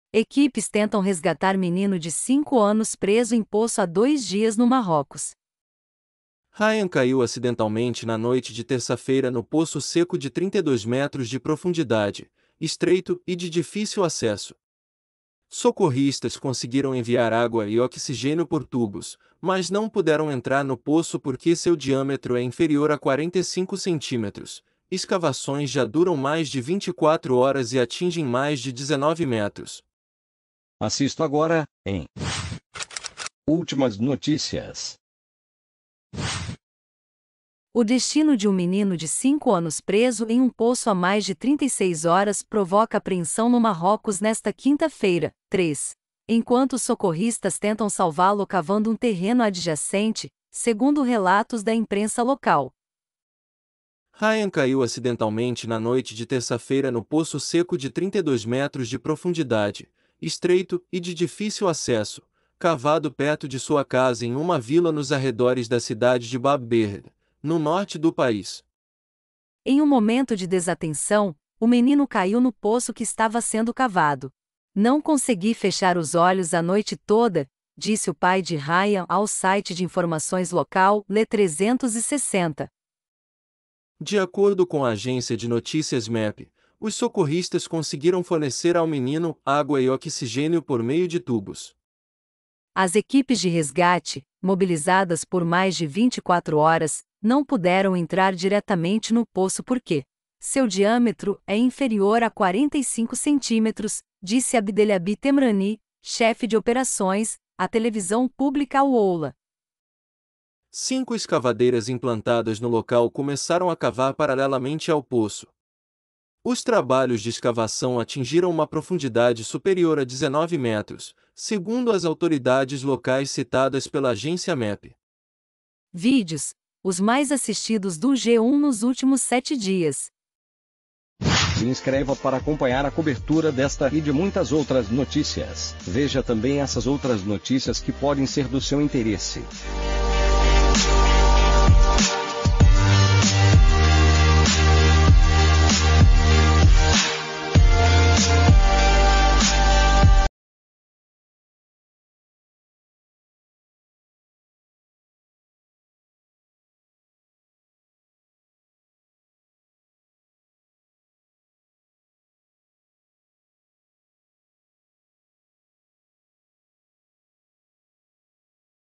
🔴 EQUIPES TENTAM RESGATAR MENINO DE CINCO ANOS PRESO EM POÇO HÁ DOIS DIAS NO MARROCOS
Rayan caiu acidentalmente na noite de terça-feira no poço seco de 32 metros de profundidade, estreito e de difícil acesso. Socorristas conseguiram enviar água e oxigênio por tubos, mas não puderam entrar no poço porque seu diâmetro é inferior a 45 centímetros; escavações já duram mais de 24 horas e atingem mais de 19 metros.

🔴 Quer saber como AUMENTAR O SEU SCORE PARA 750 OU MAIS EM APENAS 7 DIAS?

Acessado em: 3 de fevereiro de 2022 às 18:14